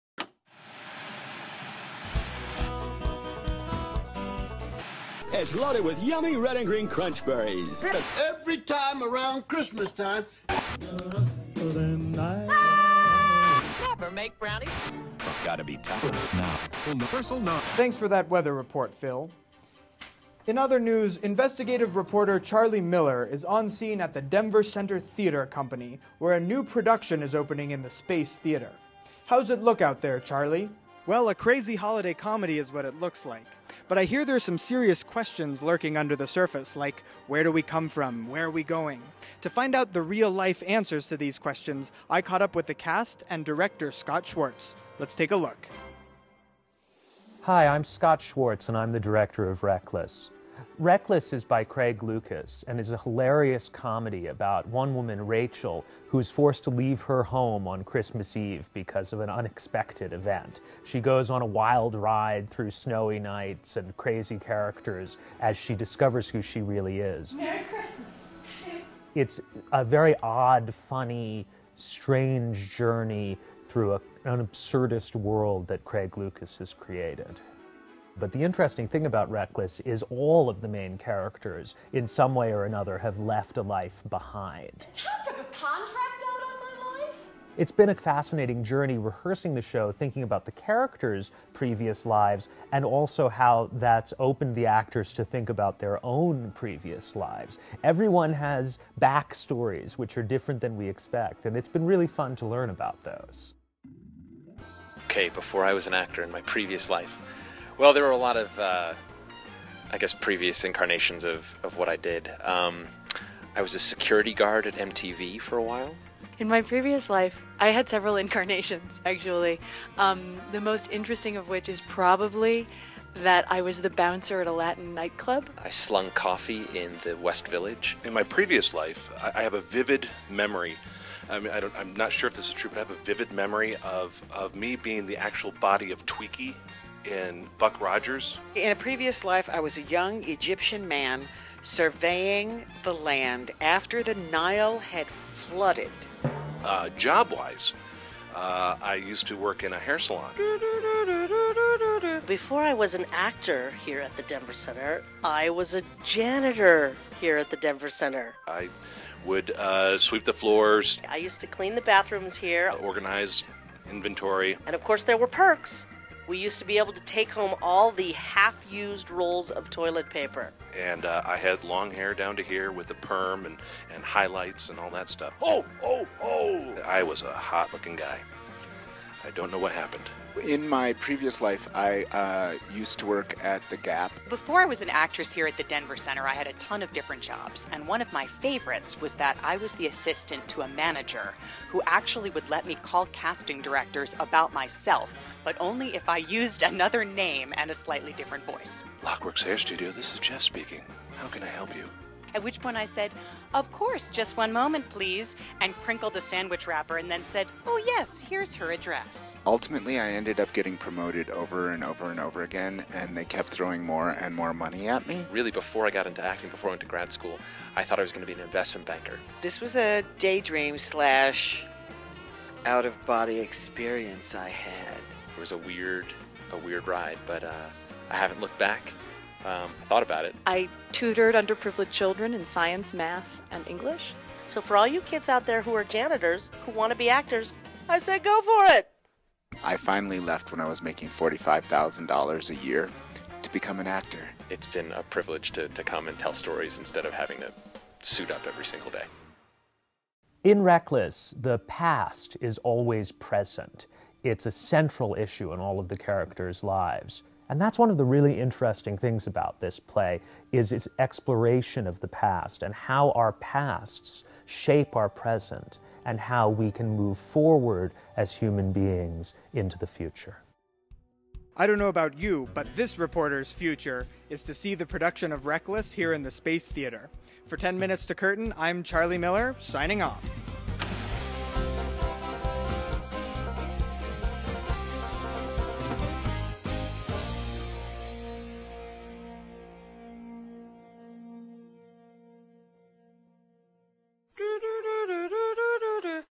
RECKLESS - 10 Minutes to Curtain
In this episode of "10 Minutes to Curtain," Director Scott Schwartz and the cast of RECKLESS discuss their past lives before they became actors.